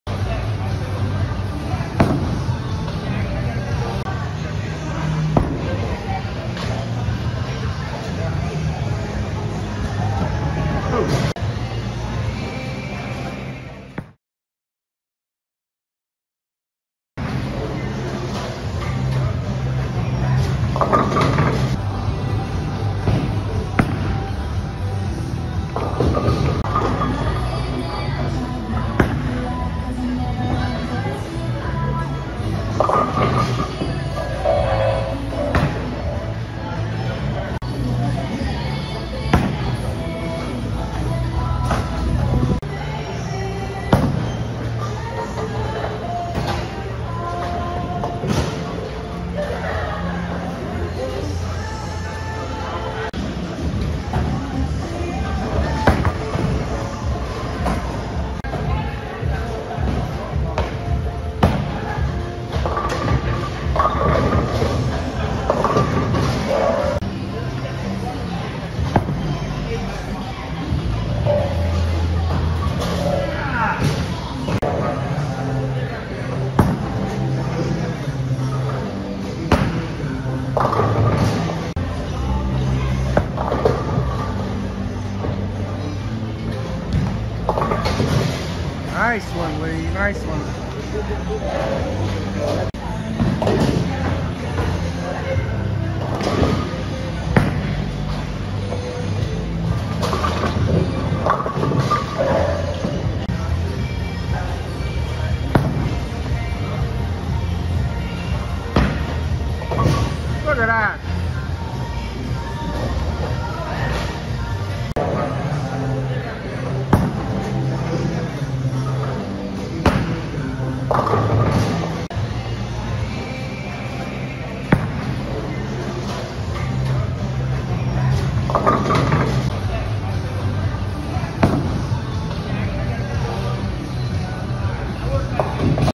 Mike's Observations movie
Mike, an excellent bowler, is trying to help me with form.  Text is my guess of what I'm doing wrong.